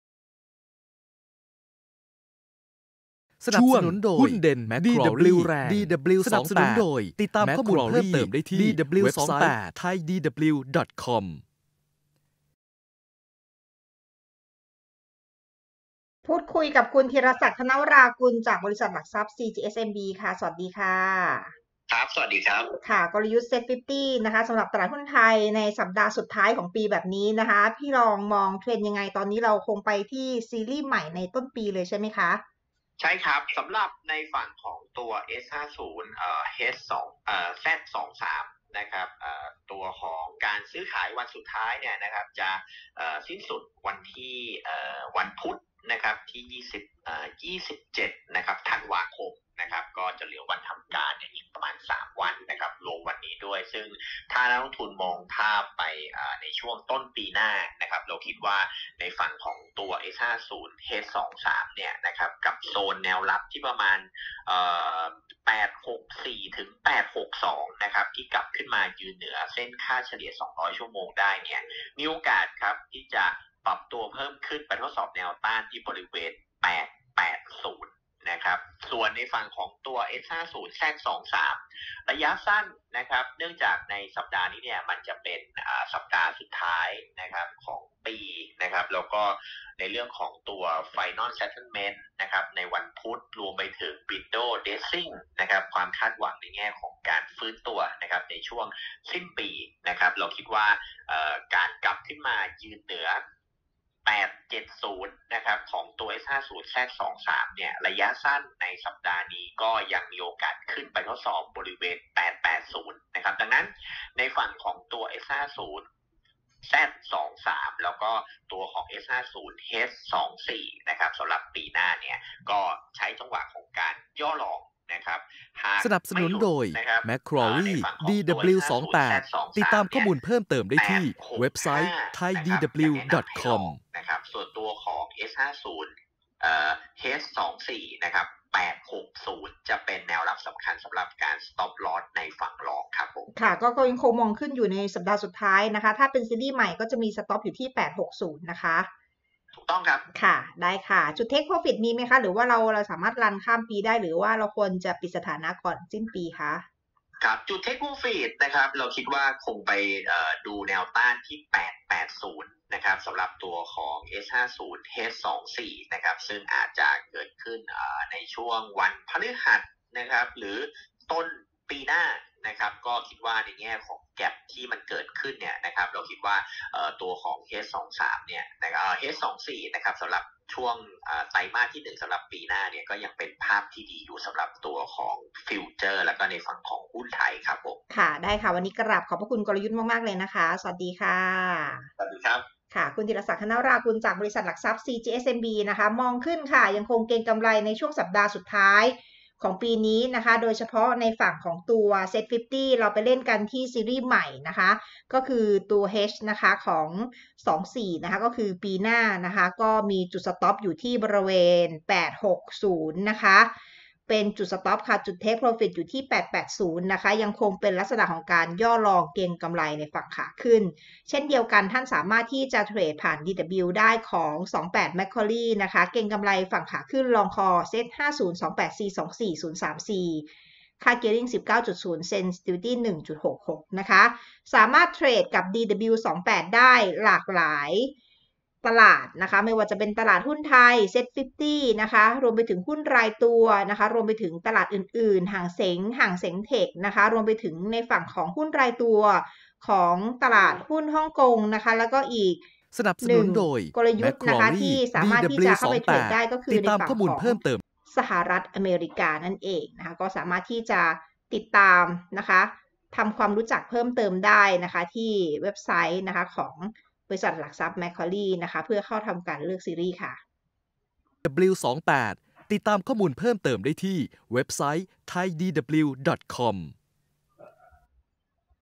มุมมอง SET50 ปลายปี 2023
คุณธีรศักดิ์ ธนวรากุล
รองผู้อำนวยการ ฝ่ายวิเคราะห์ บล.ซีจีเอส-ซีไอเอ็มบี ประเทศไทย